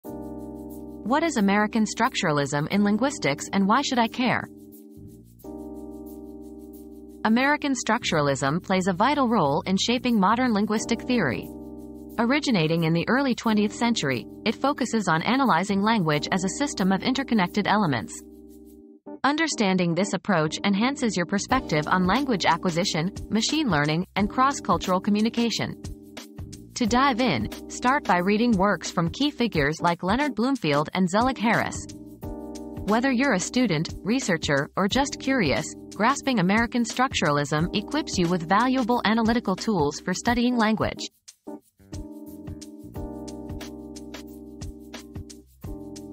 What Is American Structuralism in Linguistics and Why Should I Care?
Unlocking Language: The Power of American Structuralism • Unlocking Language • Discover the fascinating world of American Structuralism in linguistics and its impact on modern language theory. Explore how this approach analyzes language as a interconnected system and gain valuable insights into language acquisition, machine learning, and cross-cultural communication. Learn from influential figures like Leonard Bloomfield and Zellig Harris and equip yourself with essential analytical tools for studying language.